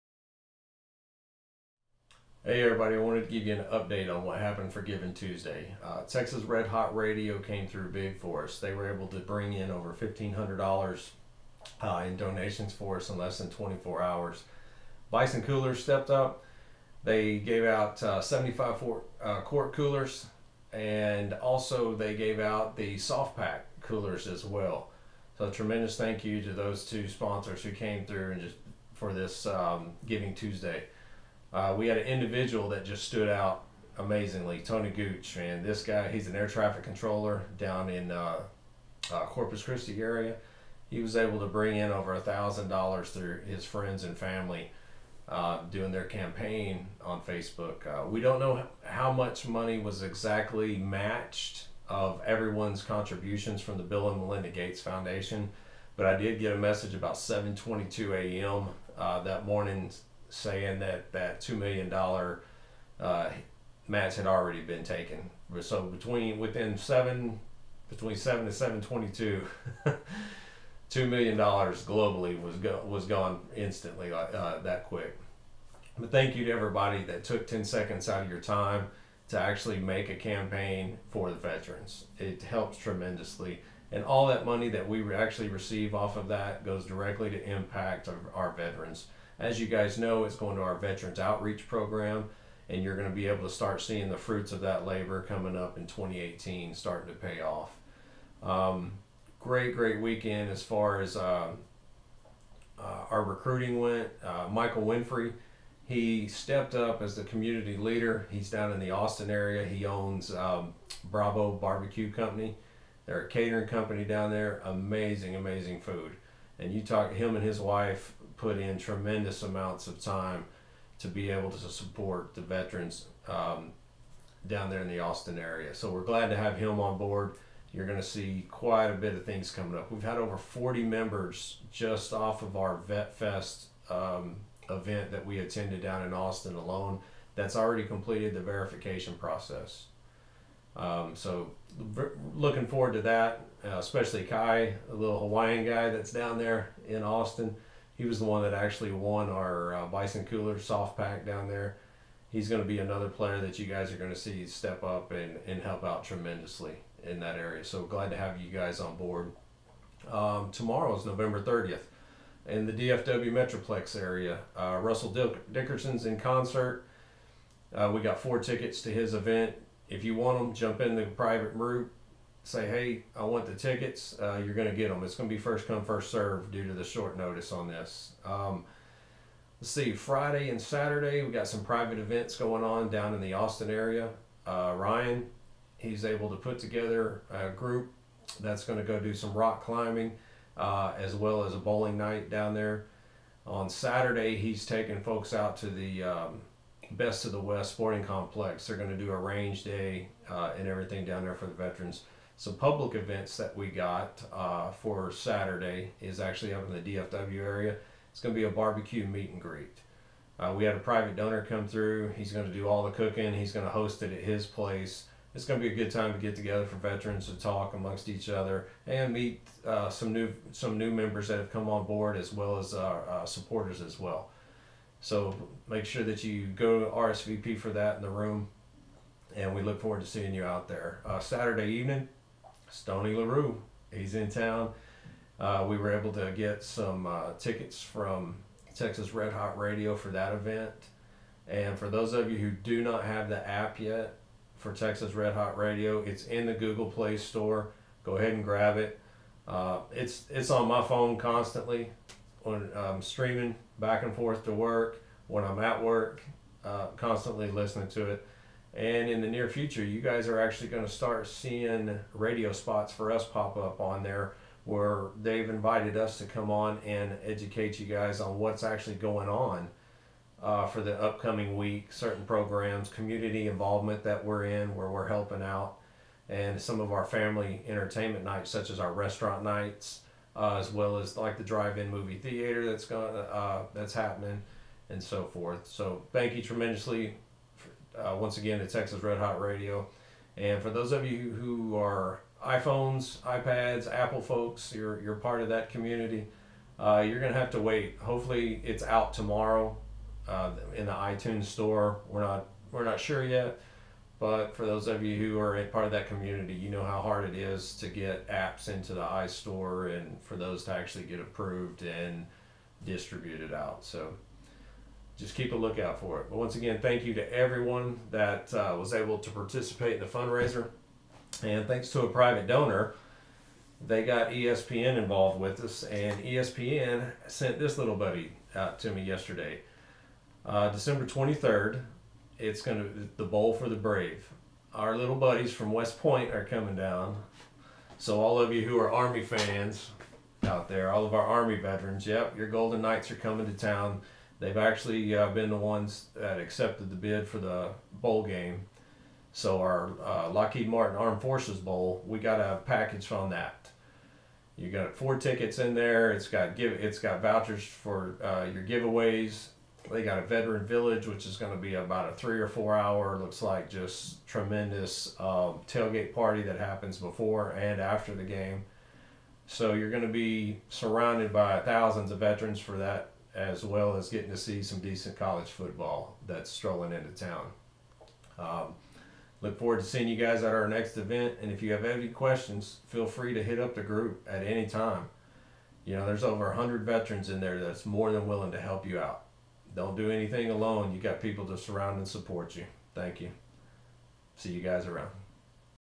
GivingTuesday Summary - 2017
Here is a summary of what happened for GivingTuesday 2017 and some upcoming events.